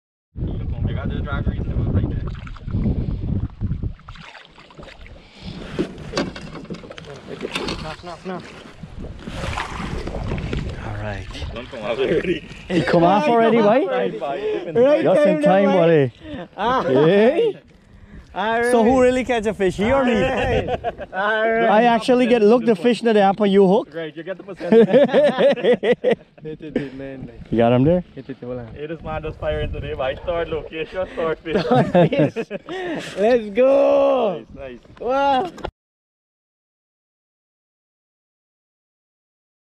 Crazy Peacock Bass almost got away!!!
Out on a fishing trip with @AlisOutdoorAdventures & @guyanaoutdoorsadventure1620